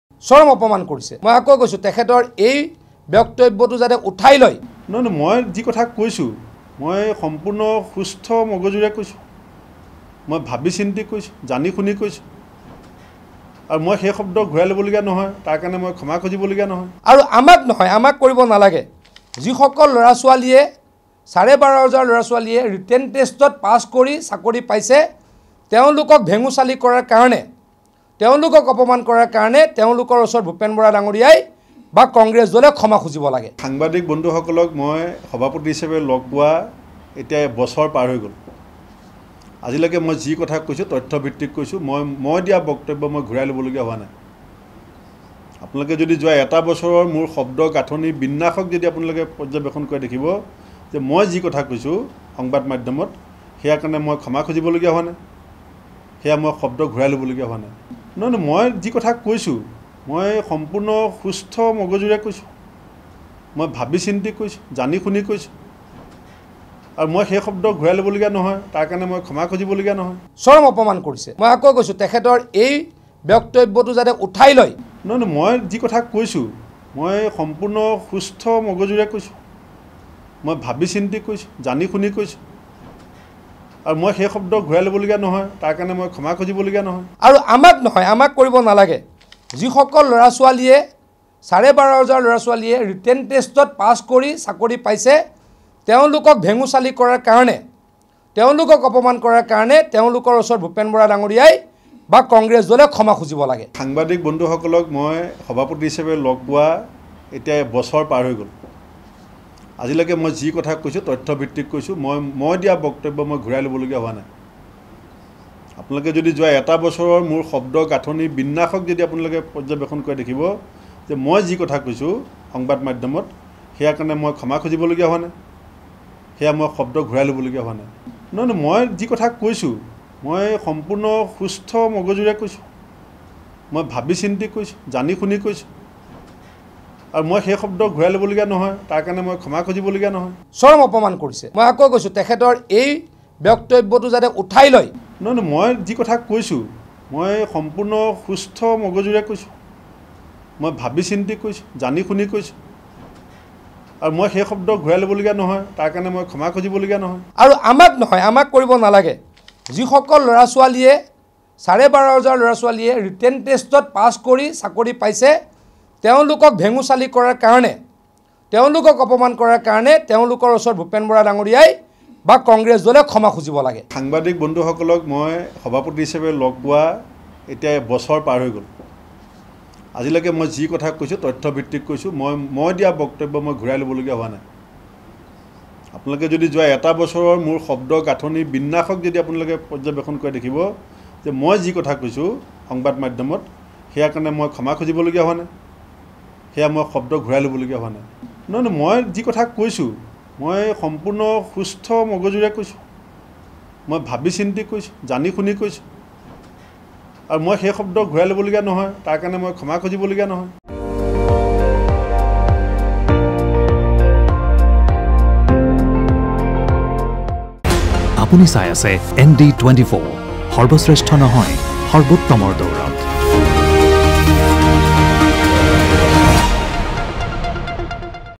কোনো কাৰণতে আৰক্ষীৰ নিযুক্তিৰ পৰীক্ষাৰ্থীক ক্ষমা নোখোজে ভূপেন বৰাই!
অসম আৰক্ষীৰ নিযুক্তিৰ পৰীক্ষাৰ্থীক ক্ষমা খুজিব লাগে ভূপেন বৰাই। মন্ত্ৰী পীযূষ হাজৰিকাৰ এই আহ্বান। পিছে ভূপেন বৰাৰ স্পষ্ট কথা - কোনো কাৰণতে ক্ষমা নোখোজে আৰক্ষীৰ নিযুক্তিৰ পৰীক্ষাৰ্থীক।